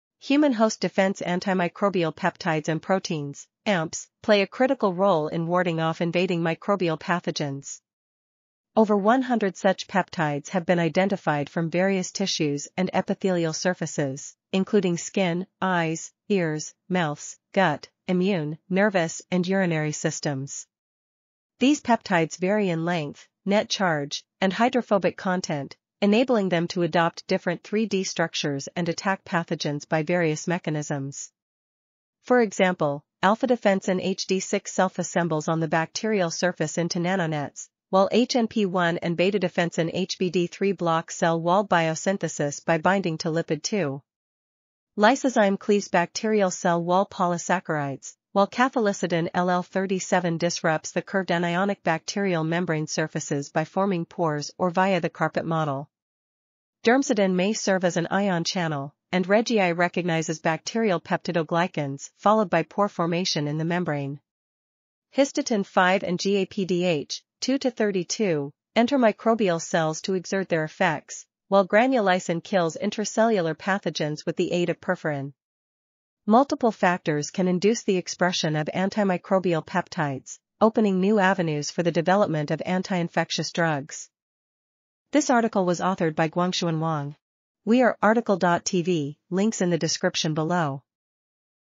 Human Antimicrobial Peptides and Proteins | RTCL.TV
### Keywords ###

### Article Attribution ###
Title: Human Antimicrobial Peptides and Proteins
Authors: Guangshun Wang
Publisher: MDPI AG
DOI: 10.3390/ph7050545

### Video Timestamps ###
0:00:00 - Summary
0:01:24 - Title
0:01:27 - Tail
0:01:31 - End